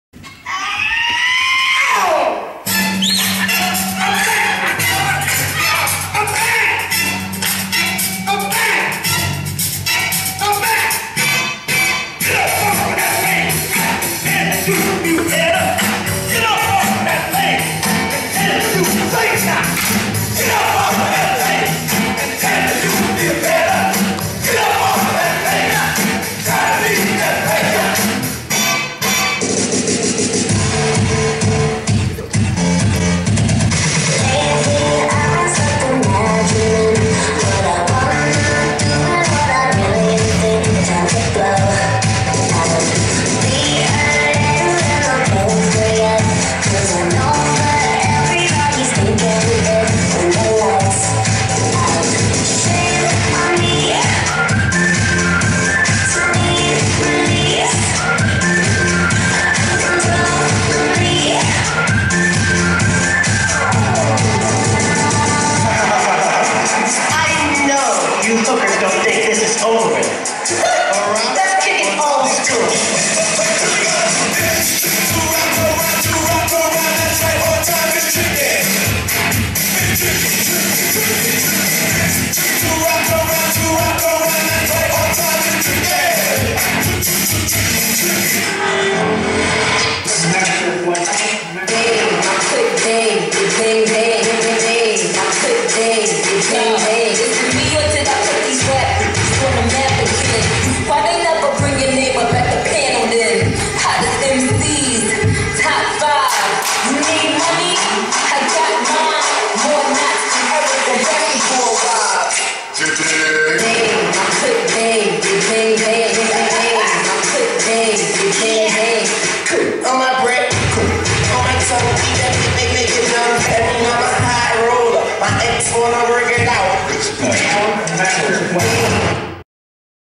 Hip Hop Piece Orientation Week Concert 2012
Part of the UCD Hip Hop Crew performing during Orientation Week! Auditions for all crews take place after Freshers Week!! :)

Choreographers:
Alesia McComish
Bianca Smith
Jessica Kelly-Hannon

Dancers:
Alesha McCallum
Alesia McComish
Dereck Gombedza
Elayne Ruane
Firas Al Shakarchi
Jessie Kelly-Hannon
Rebecca Martin